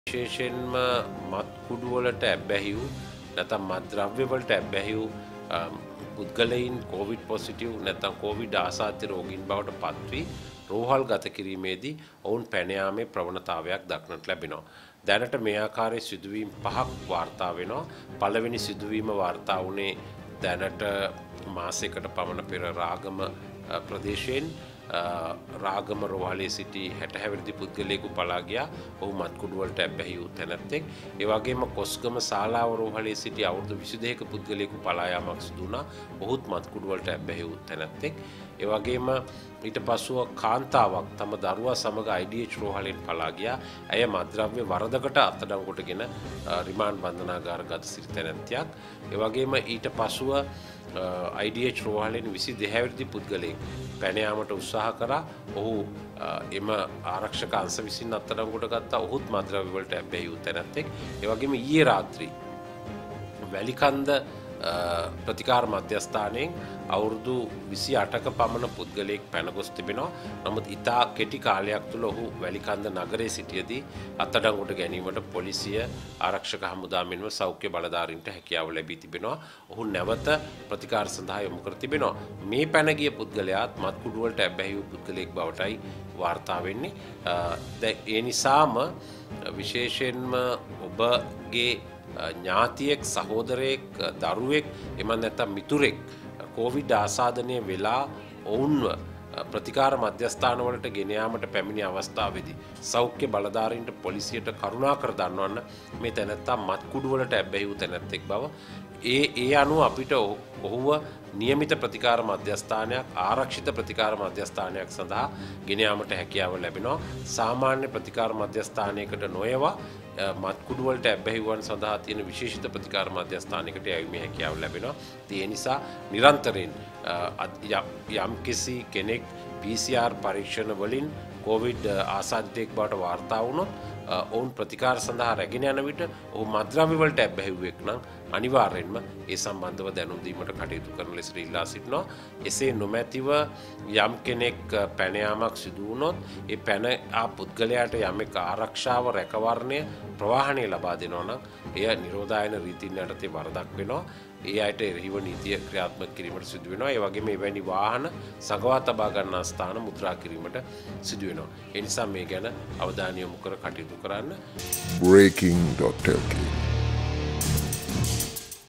ඊයේ දිනයේ වැලිකන්ද කොවිඩ් රෝහලෙන් පැන ගිය පුද්ගලයාට සිදු වූ දේ | Breaking.lk
ඊයේ දිනයේ වැලිකන්ද කොවිඩ් රෝහලෙන් පැන ගිය පුද්ගලයාට සිදු වූ දේ